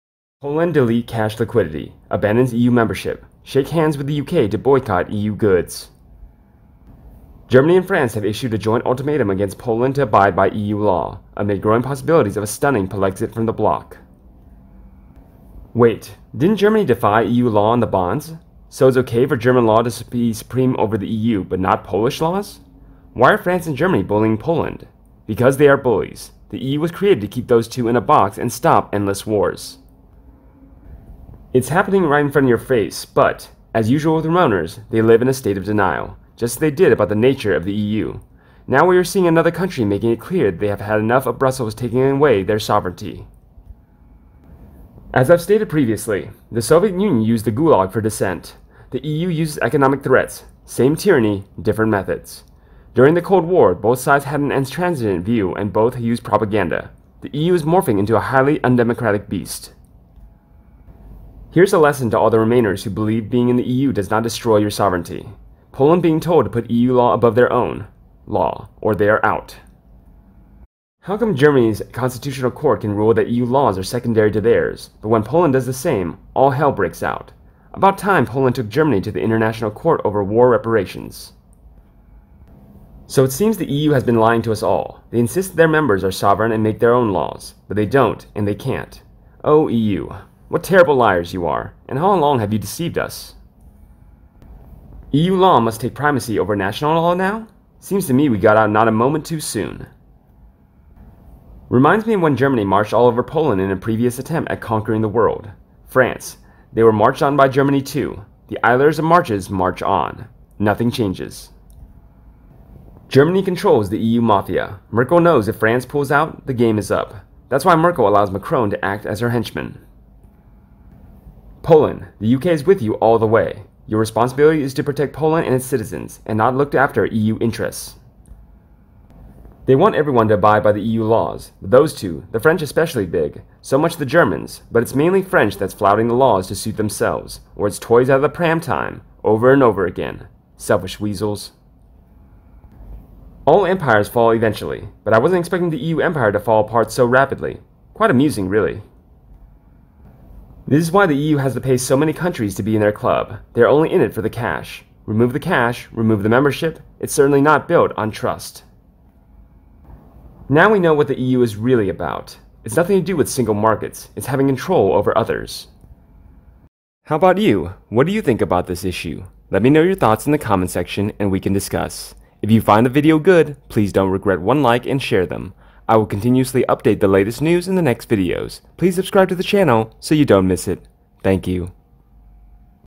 Poland DELETE Cash Liquidity abandons EU membership - shake hands with the UK to BOYCOTT EU goods
GERMANY and France have issued a joint ultimatum against Poland to abide by EU law, amid growing possibilities of a stunning Polexit from the bloc.

Wait. Didn't Germany defy EU law on the bonds? so it's ok for German law to be supreme over the EU but not Polish law? Why are France and Germany bullying Poland? Because they are bullies, the EU was created to keep those two in a box and stop endless wars.